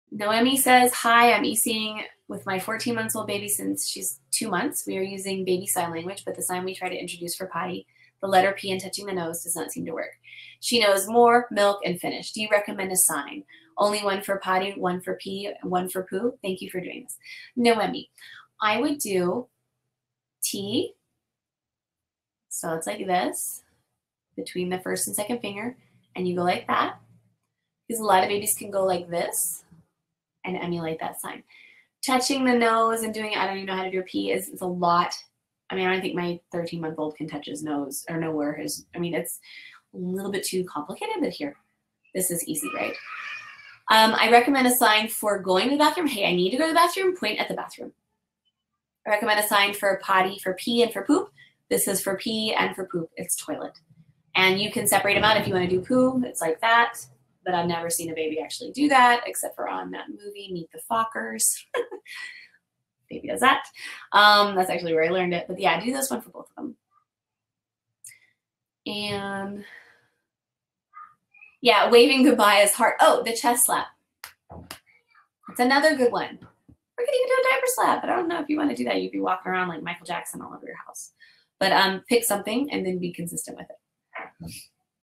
Do you recommend a sign? Only one for potty or one for pee and one for poo?
Today's question comes from a parent who is working on sign language with their baby and is wondering what signs I recommend for pee and poop and if there are alternatives if one isn't working! In today's video I'll talk about what you can do if your baby isn't getting the hang of a particular sign, and how as long as there is communication between the two of you, that is more important than getting it exactly right. For more information on sign language, check out my blog post Teaching Baby to Signal

Original Question:

"Hi! I'm ECing with my 14 months old baby since her 2 months. We are using baby sign language but the sign we try to introduce for potty (letter P and touching the nose) do not seem to work... She know "more" , "milk" and "finish". Do you recommend a sign? Only one for potty or one for pee and one for poo? Thank you for doing this!"